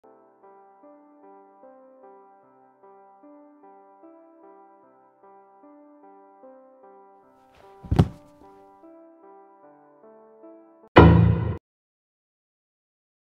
Der kleine Geist (Animation)
Ich habe eine kurze 2D Animation erstellt, du würdet mir sehr helfen wenn ihr mir ein kurzes Feedback dalassen würdest.

Die Sounds wurden von zur Verfügung gestellt.

Dieses Video dient mir in erster Linie als Basis für die 3D-Animation die ich im laufe der Zeit erstellen möchte, aber bis es so weit ist dauert es noch eine weile.